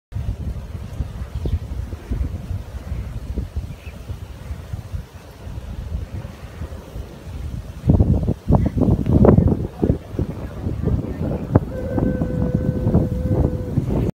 Mendes Horse Ranch
This short video shows views of Haleakala and the Pacific Ocean from the cliffs at the Mendes Horse Ranch on Maui.  It was taken just before we rode horses for a couple of hours.

These guys have a nice office view each day.